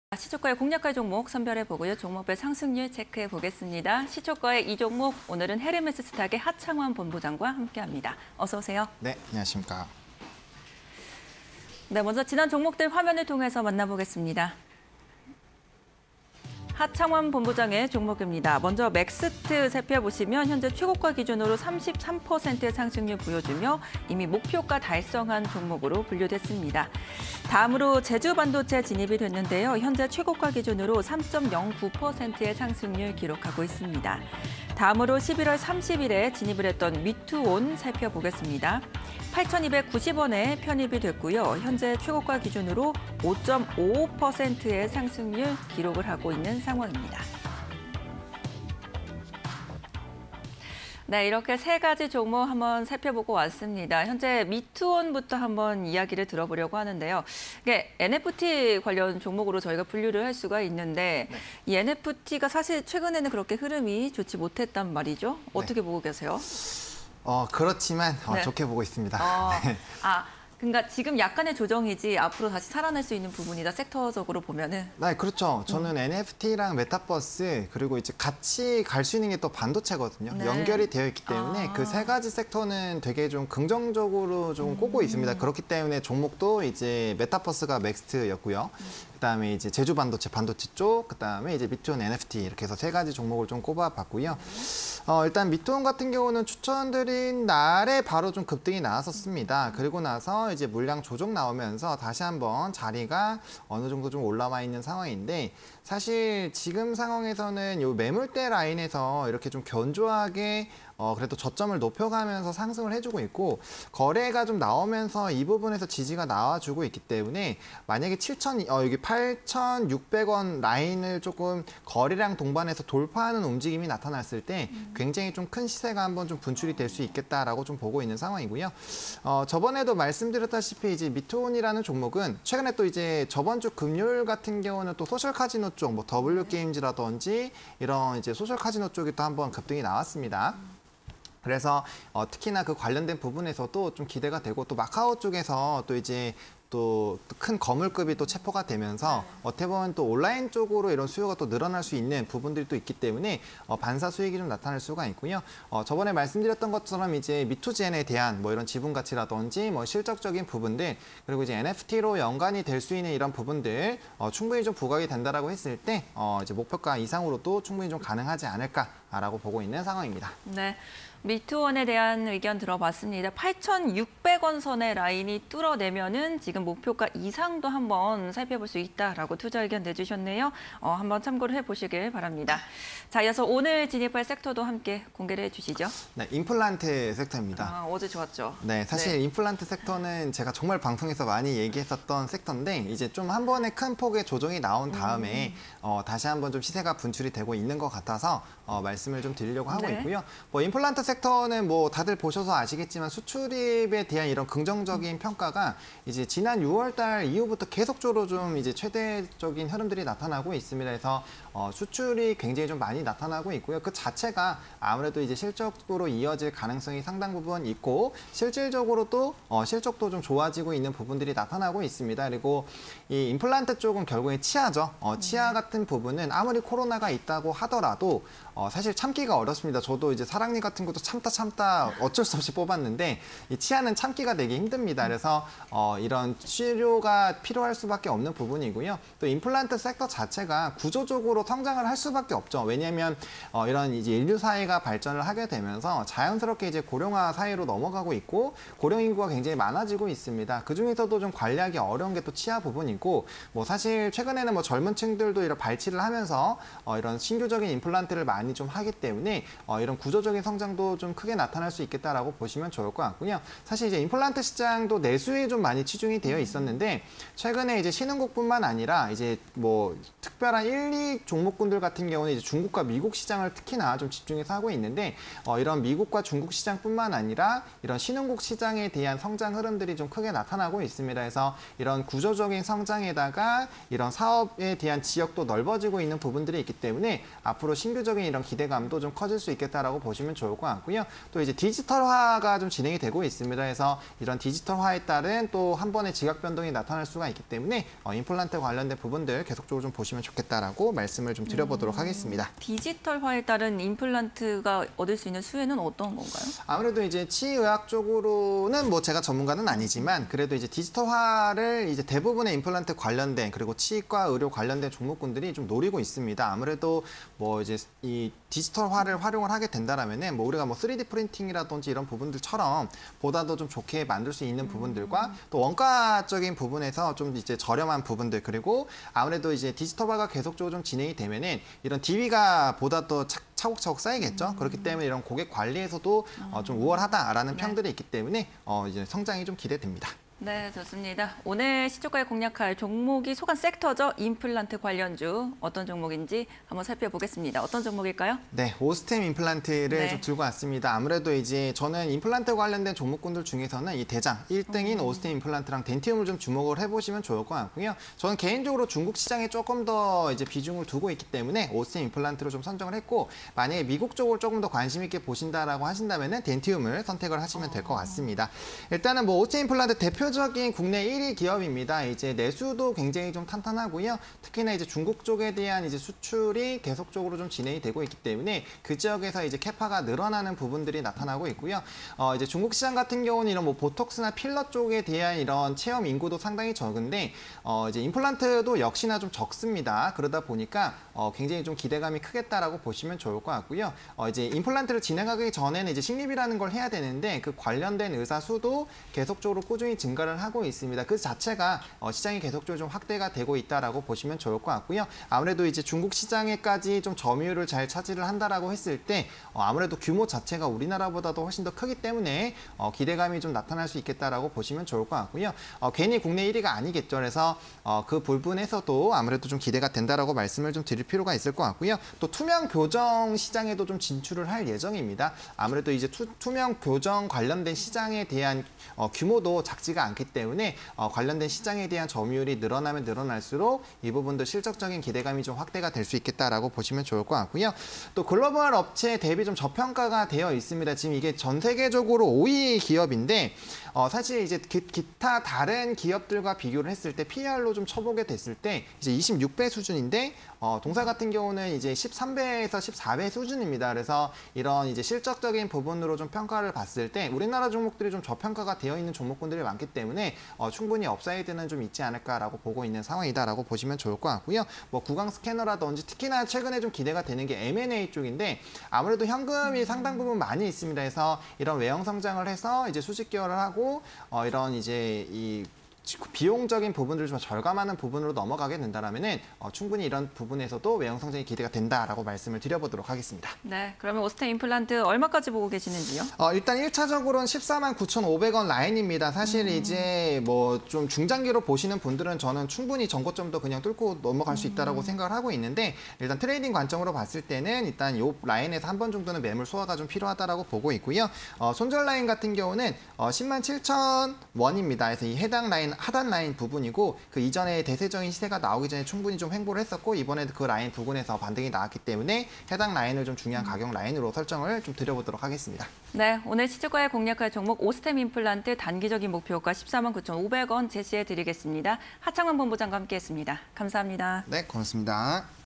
오스템임플란트(048260)... 임플란트 관련주_시초가에 이 종목 (20211207)
임플란트 관련주
- 3분기까지 월별 지속적 성장 유지
- 전년동월대비 수출액 36.4%↑
- 4분기에도 성장세 유지 기대
- 수출 데이터 호조… 실적 기대감 확대
- 고령화 시대… 수요 지속 전망
- 신흥국 중심 임플란트 업종 구조적 성장세

오스템임플란트(048260)
- 글로벌 임플란트 시장 점유율 5위
- 내년 中 시장 ‘투명교정사업’ 진출
- 美 시장 진출 효과 본격 기대
- 글로벌 경쟁 업체 대비 저평가
- 디지털화에 따른 시장 선점 기대
- M&A에 따른 외형 성장 기대